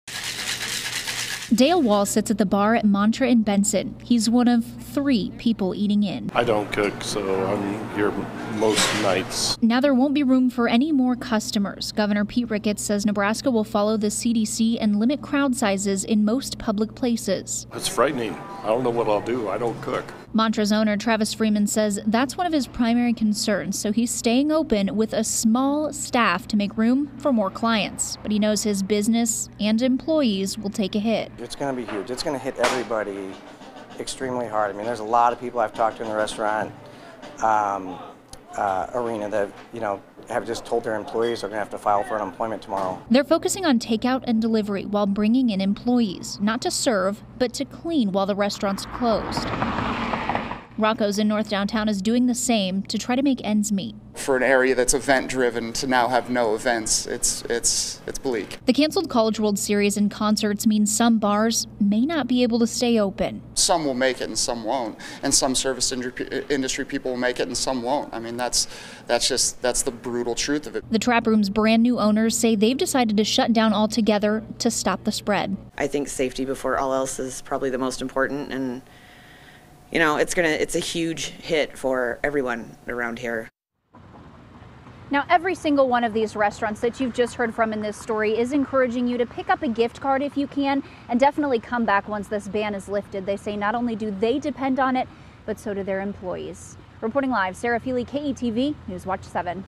Businesses, staff face financial strain with limited crowds
Covid-19 impacting local businesses and staff